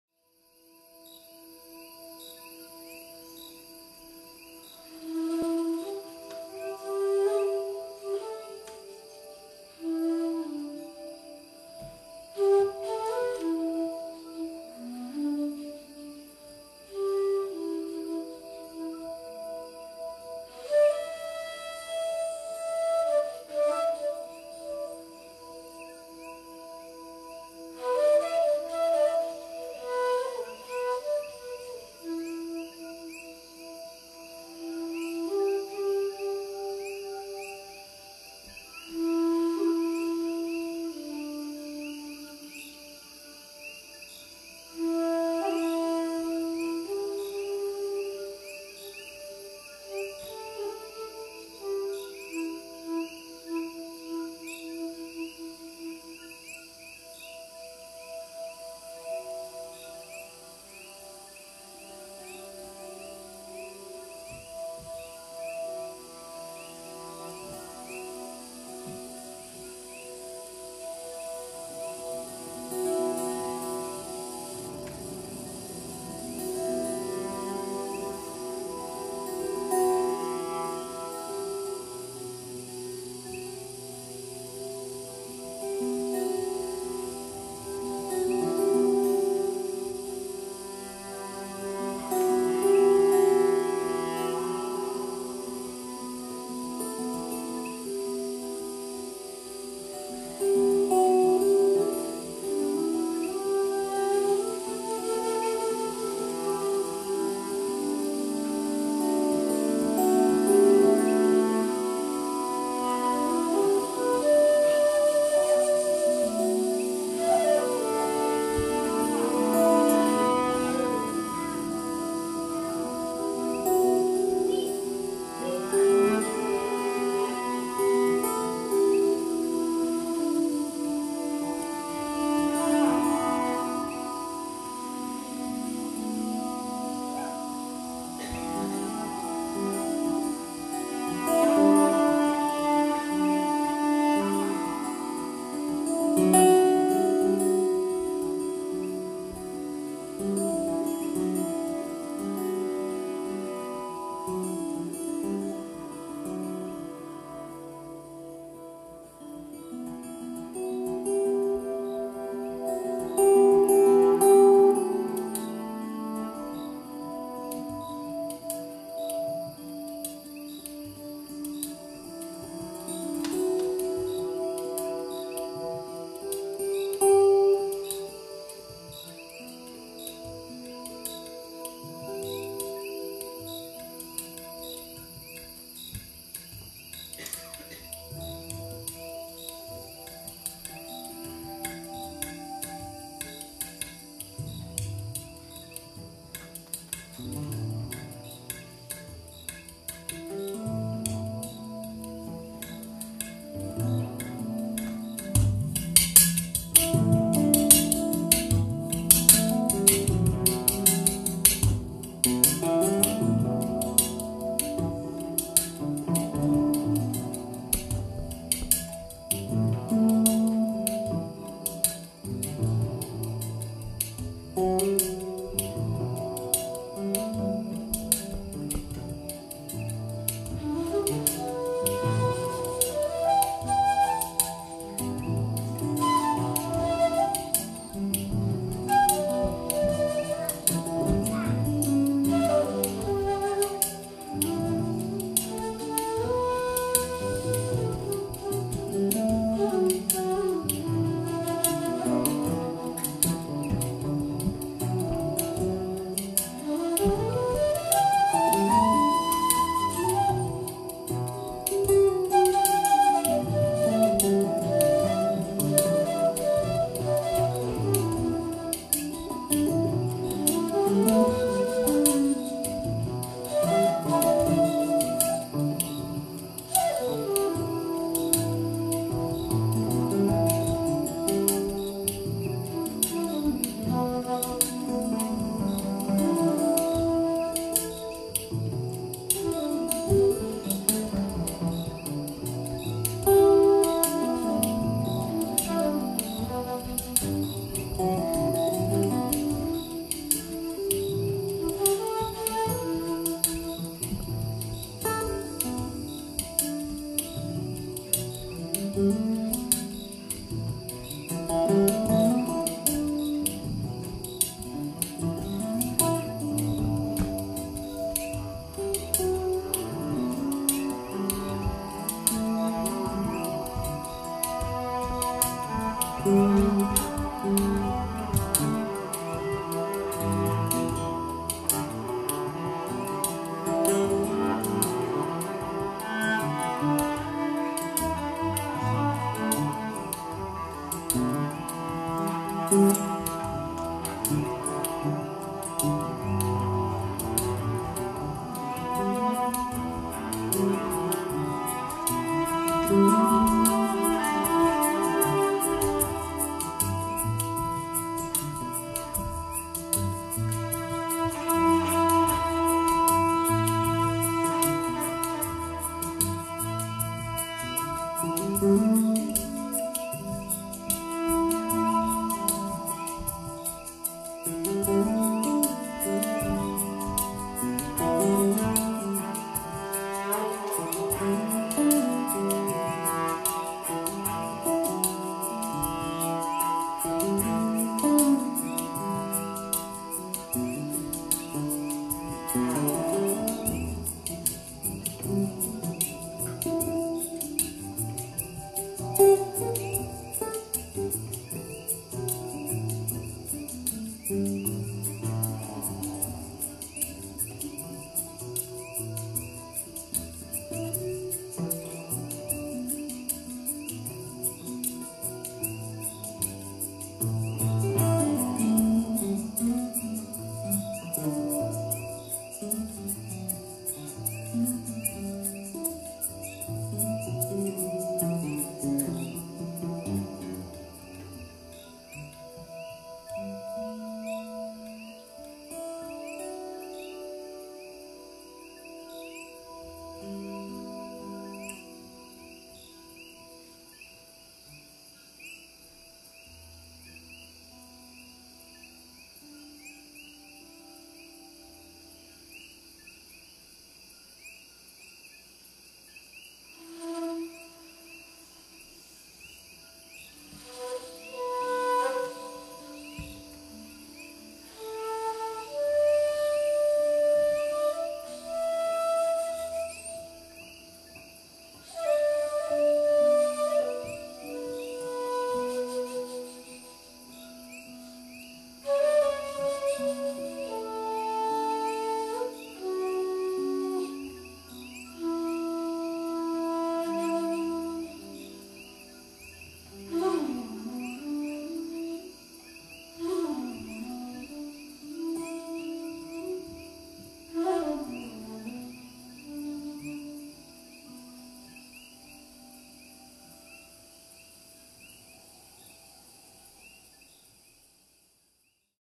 Talking with Trees 2
Isabel, Chris and I at the Raga Room July 23/2017.